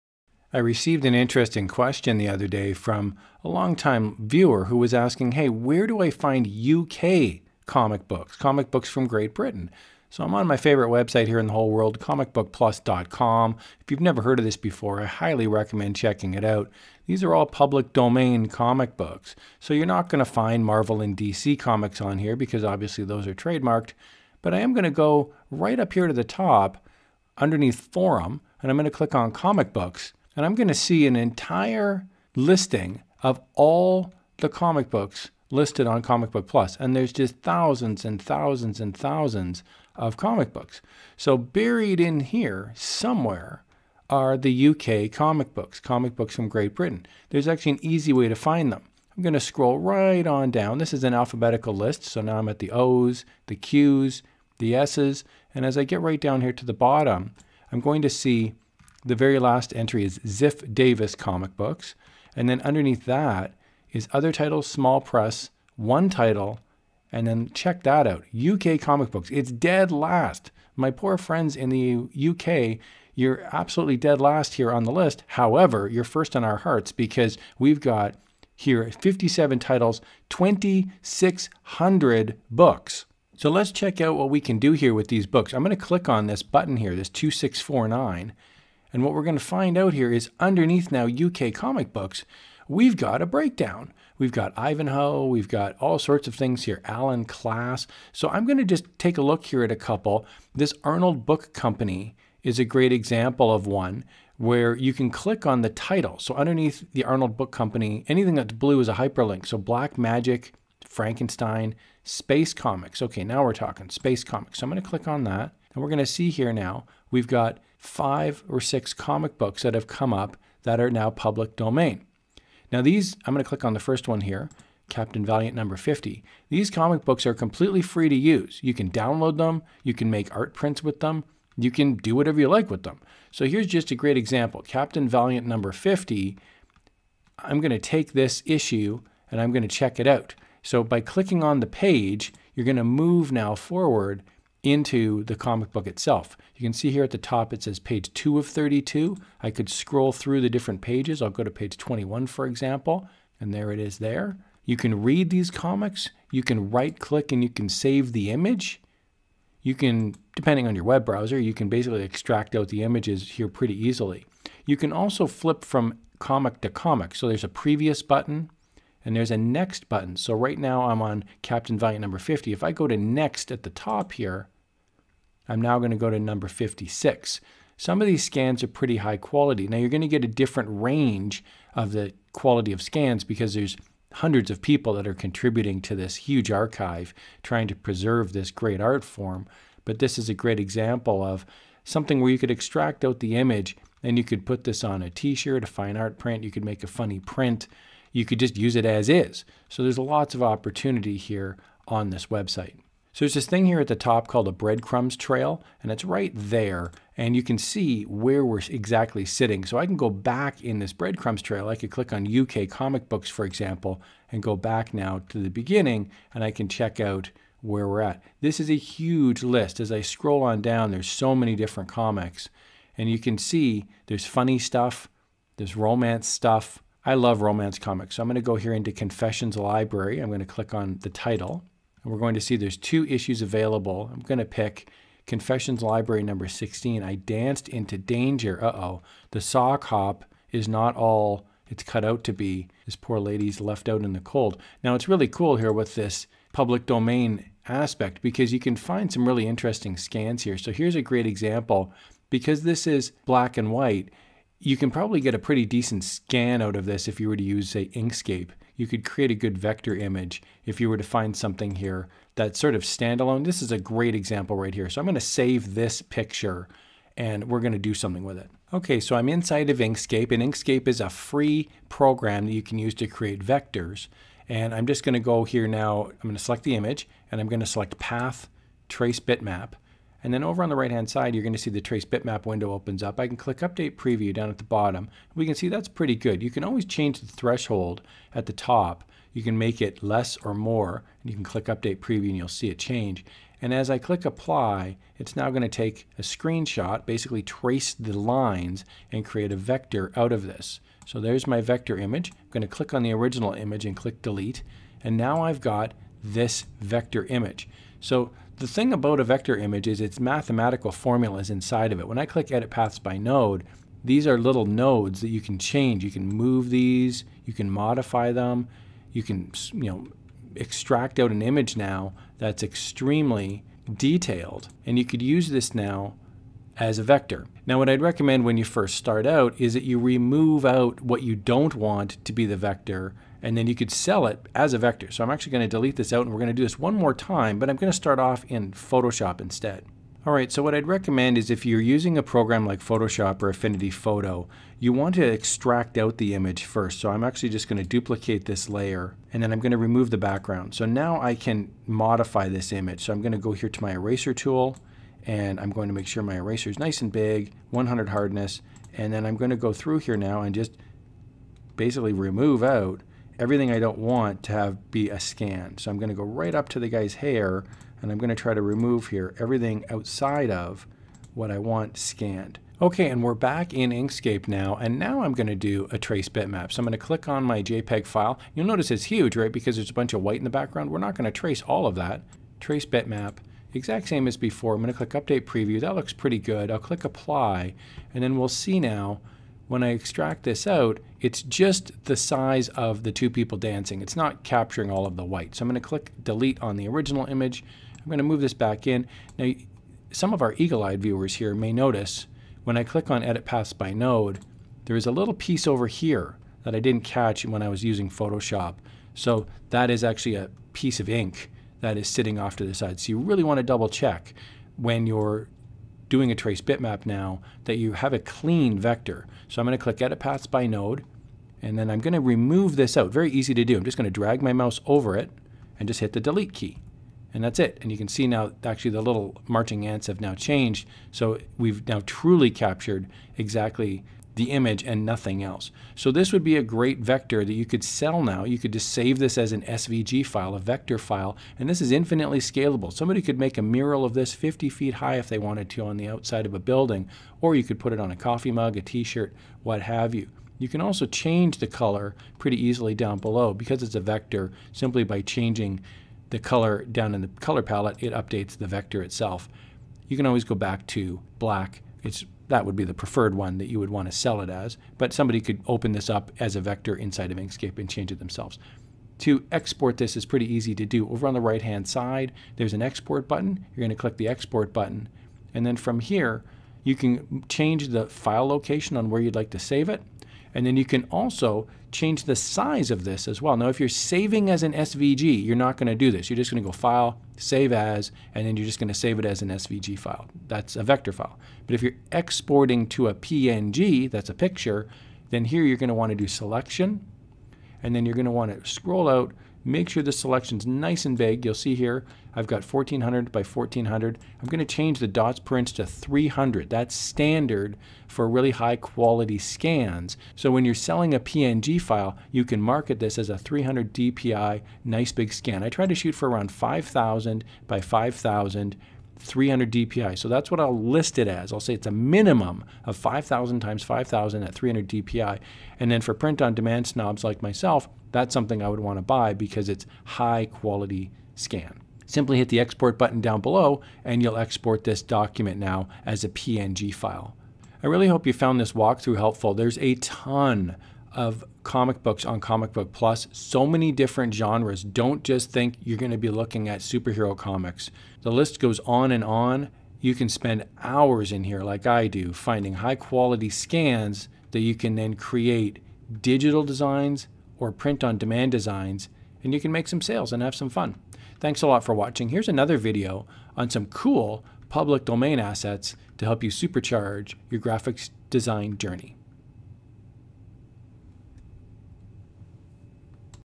FREE & LEGAL United Kingdom Comics
I love vintage comic books! In this video, I cover comic books from the United Kingdom—here's how to find them! I also do a walkthrough in both Inkscape and Photoshop on how you can extract out an image for use. You can sell the digital file, or you can sell print-on-demand products using these awesome pictures. The best part?

These books are free and contain copyright-free images that are great for creating designs for sales on print on demand websites like Etsy, Merch By Amazon or Redbubble.  In this video, I do a walkthrough of the website comicbookplus.com and also showcase a few ideas on what you can actually make with these vintage designs.